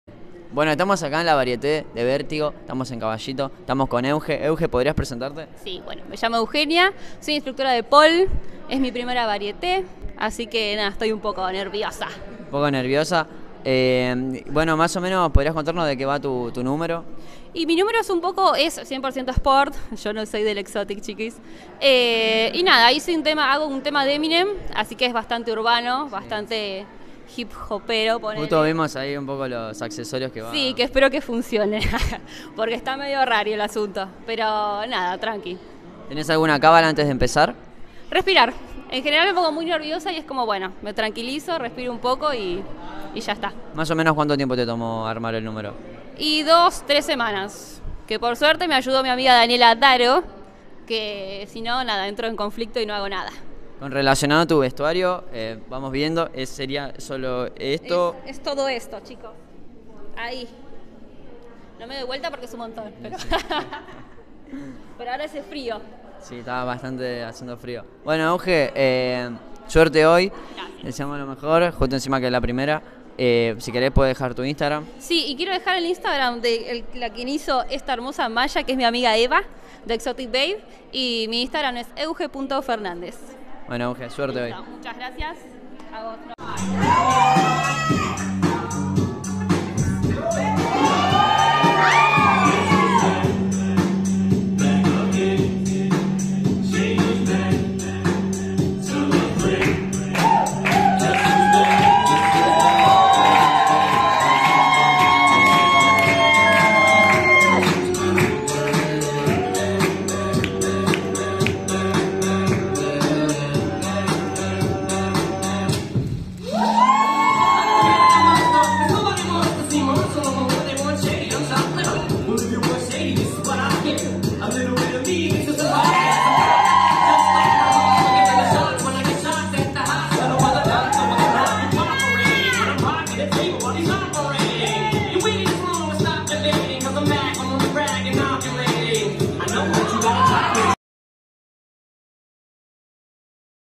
✨Cobertura Variete - Eugenia Fernández
Eugenia Fernández participa por primera vez en la Variete, nos cuenta como se preparo.
Armo una coreografíade junto a su entrenadora Daniela Adaro de Pole Sport con música urbana.